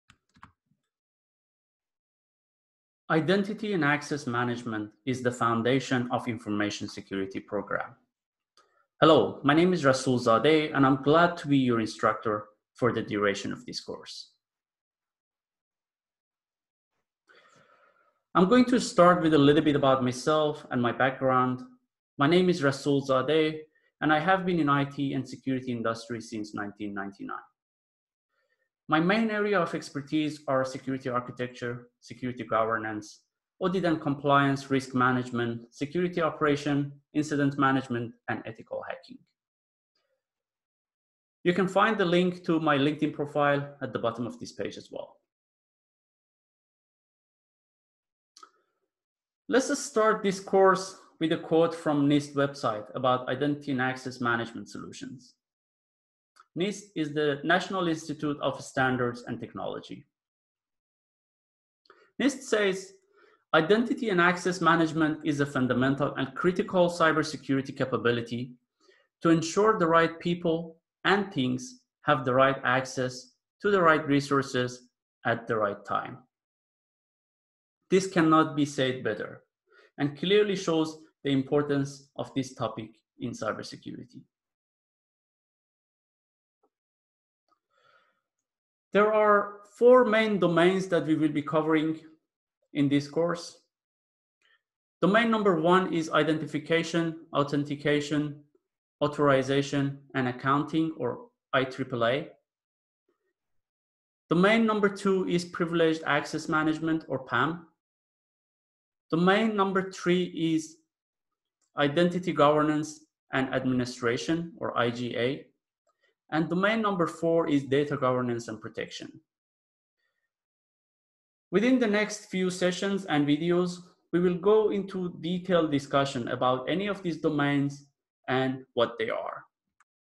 Identity and Access Management Training Course - Introduction
In this Identity and Access Management Training course, you will learn about the following:
Identity and Access Management (IAM)
Identification, Authentication, Authorization and Accounting (IAAA)
Identity Governance and Administrations (IGA)
Privileged Account Management (PAM)
Identity Lifecycle management (ILM)
Data governance and protection